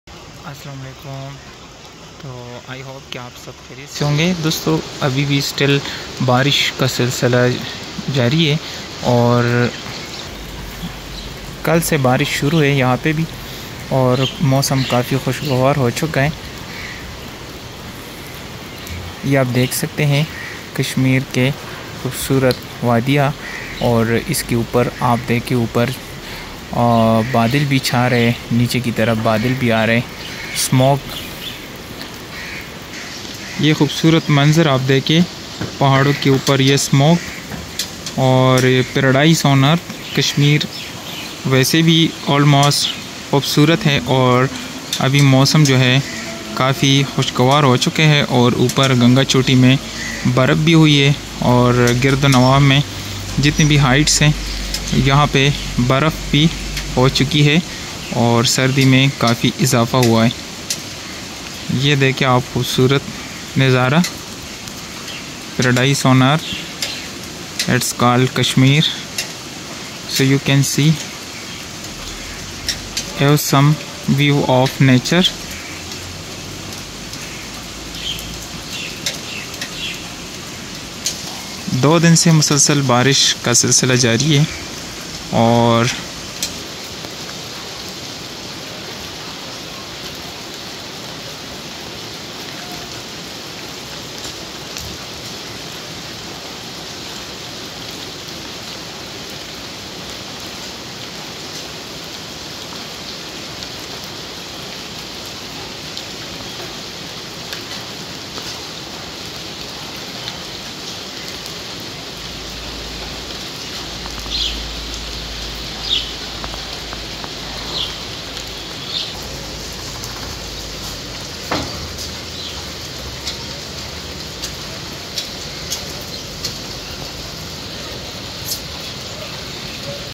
Endless Rainstorms in Kashmir | GSK Adventure's Wet and Wild Journey
join us on an unforgettable adventure through the relentless rainstorms in the stunning Kashmir region. GSK Adventure takes you on a journey to experience the beauty and challenges of this waterlogged paradise. Get ready to be soaked in the magic of Kashmir's never-ending rain!